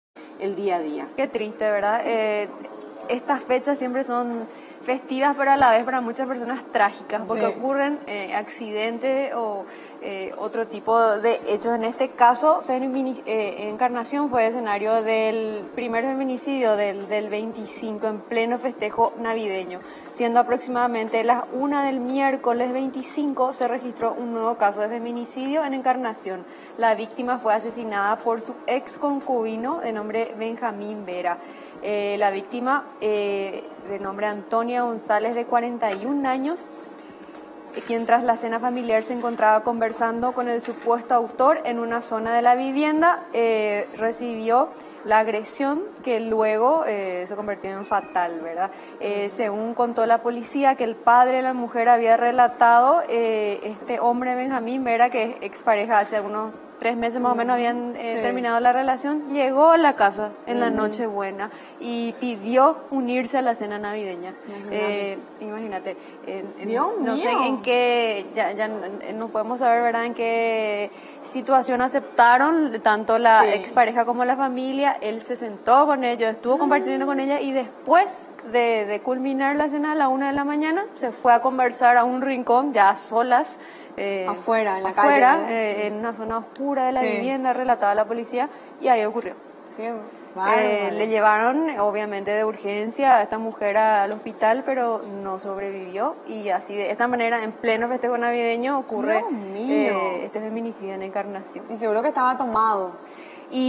Feminicidio en Encarnación
Feminicidio en pleno festejo navideño en Encarnación.
Fuente: GEN.